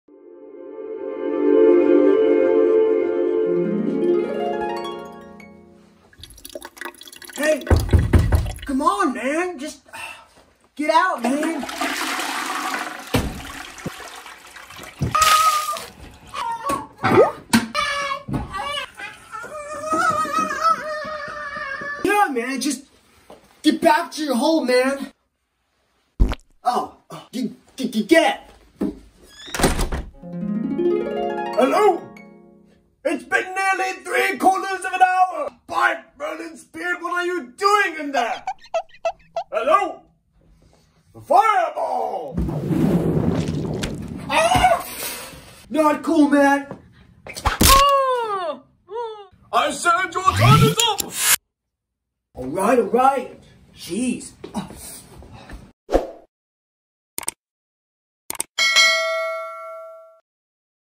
A Wizard, a Halfling, and a Ranger | 1 Toilet | Who wins?
When a wizard, halfling, and ranger all want to heed the call of nature with only one bathroom between them, things get…interesting.